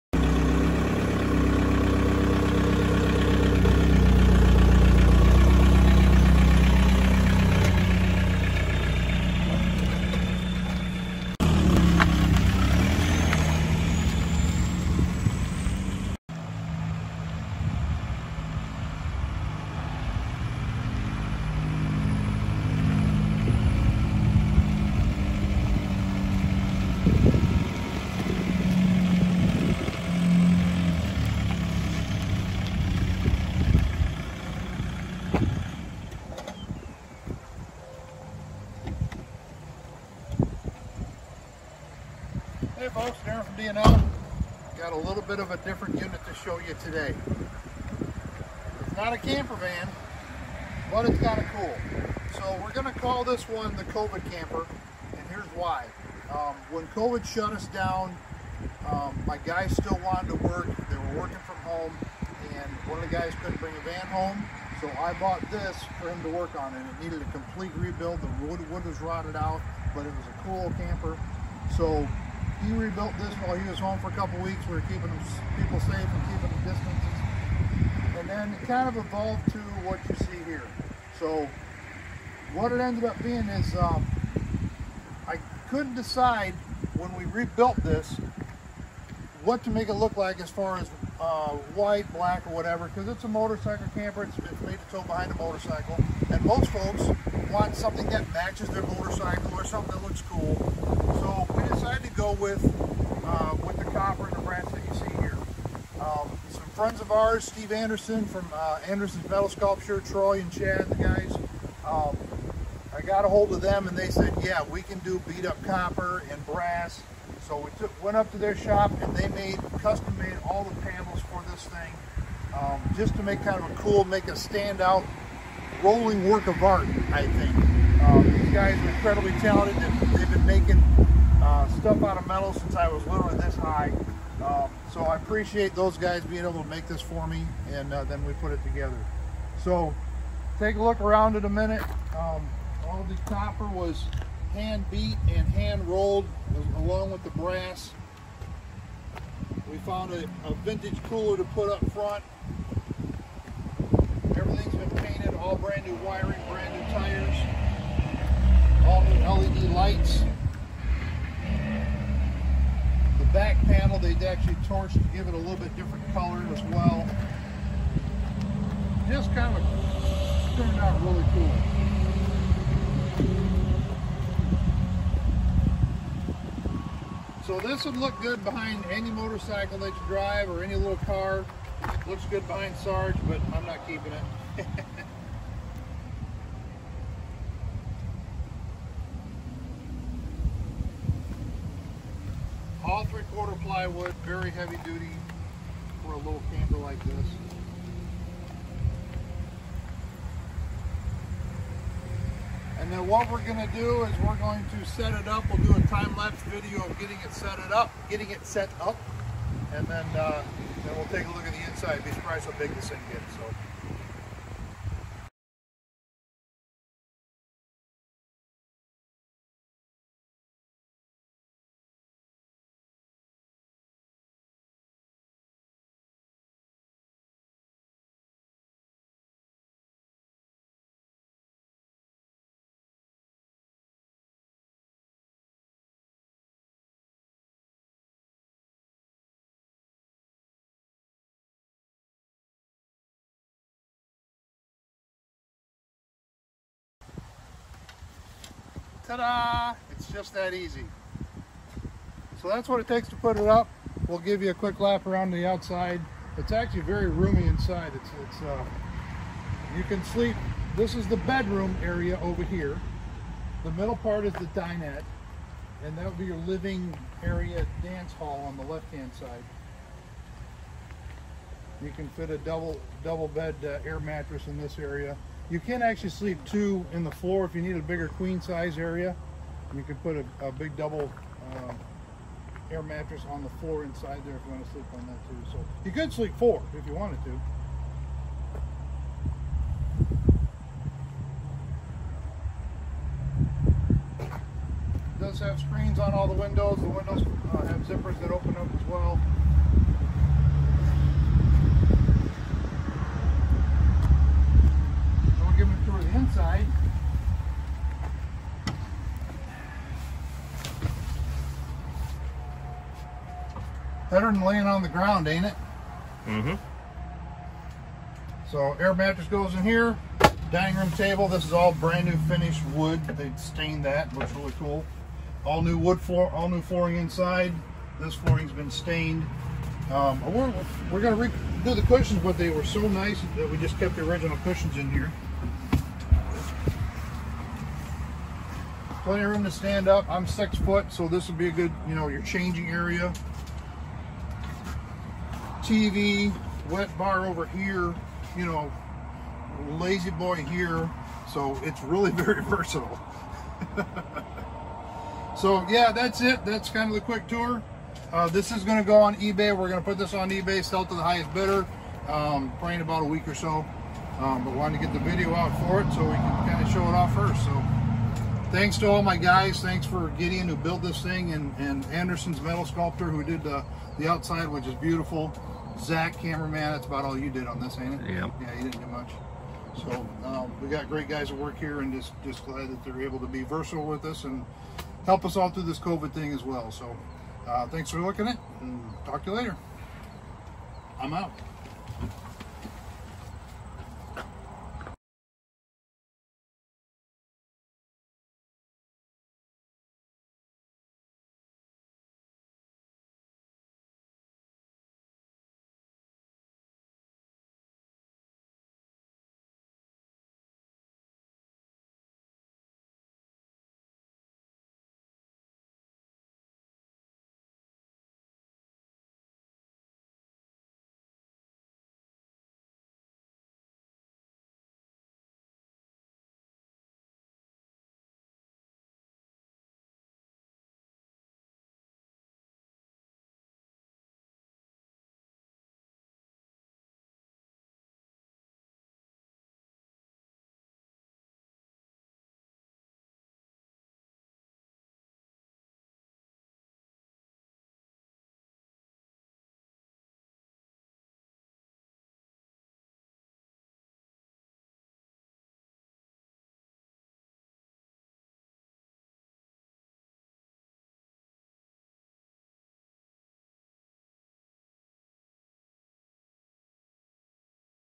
Motorcycle Pop Up Camper Trailer. the "Covid Camper"
SOLD!
This Very cool and fully restored Mini Pop Up Camper Trailer will be going up for sale on a 7 day Ebay auction and sold to the highest bidder. I will post the link here when it goes live as well. We hope to have the auction start on Sunday evening Sept 20th, 2020. Please share this with all of your Motorcycle Buddy's.